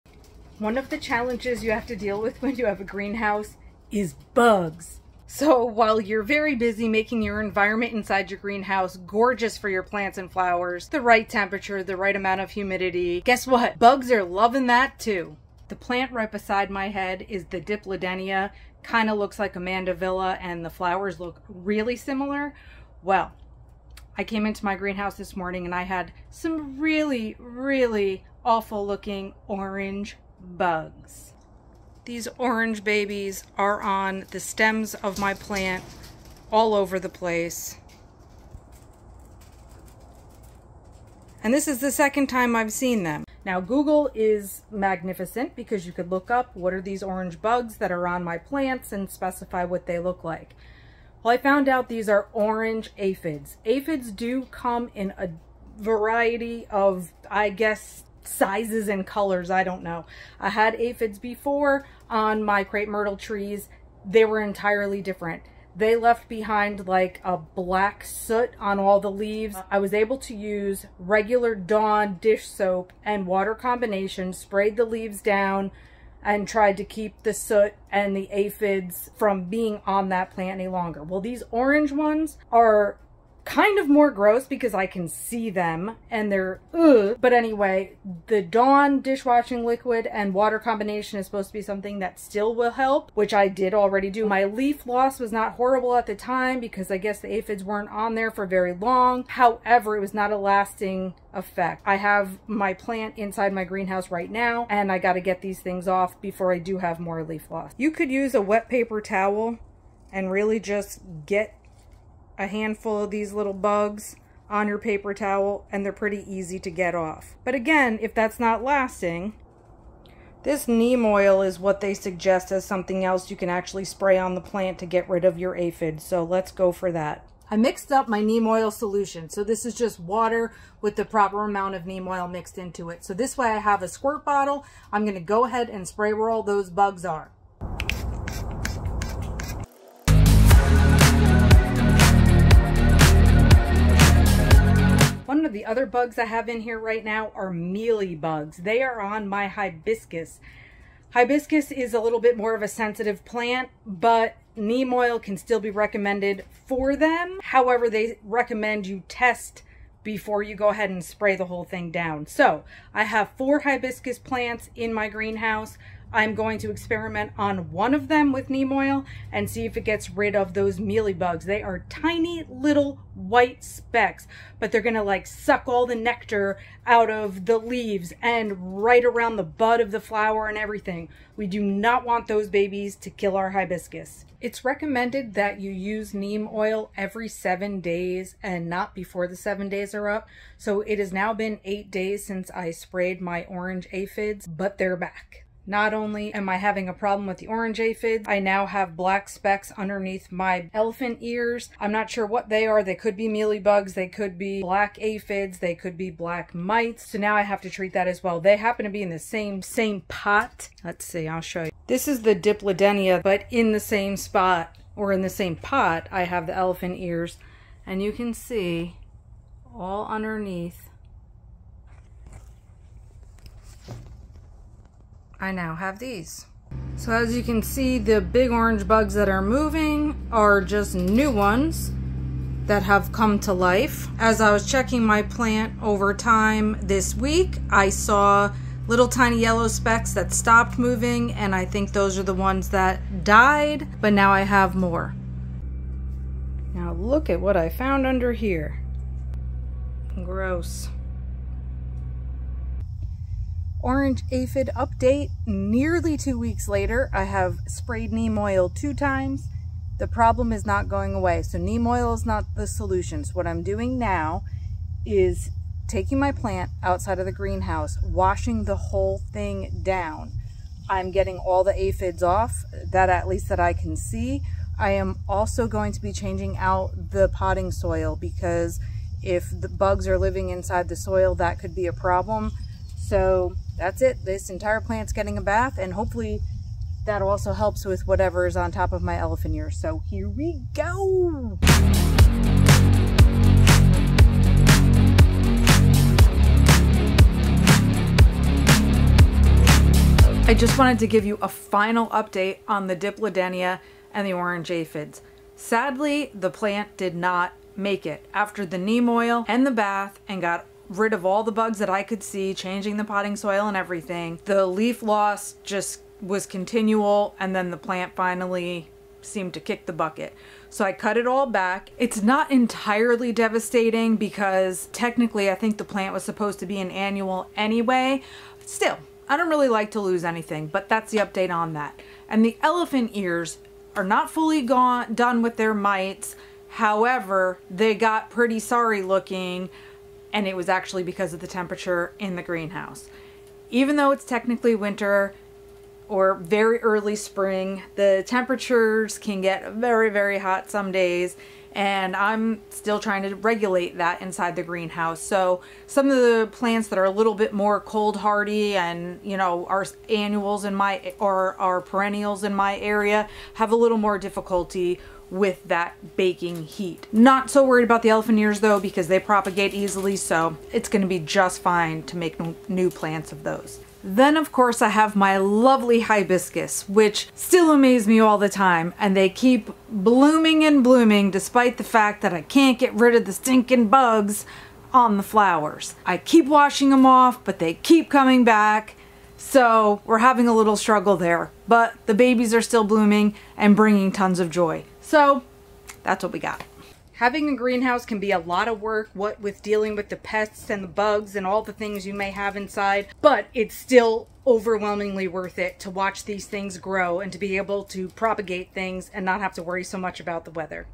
Bug Infestation | Greenhouse Challenges | Aphids
All I wanted was a greenhouse in my backyard that would house my beautiful plants, and make everything look stunning.  Turns out that greenhouse challenges are real, and there's no escaping them.

My sweet backyard greenhouse got a bug infestation.  Aphids, mostly.  A few varieties of aphids, actually.  This bug problem was partially a fail that I want to share, and partially a success.

I guess I was so focused on growing beautiful plants and flowers that I didn't think about the many difficulties that go along with having a greenhouse in my Carolina garden. I'll get into some more of those difficulties with you moving forward so you can hopefully learn from my mistakes without having to make them on your own!  Also, for more on gardening tips in North Carolina, stick with me!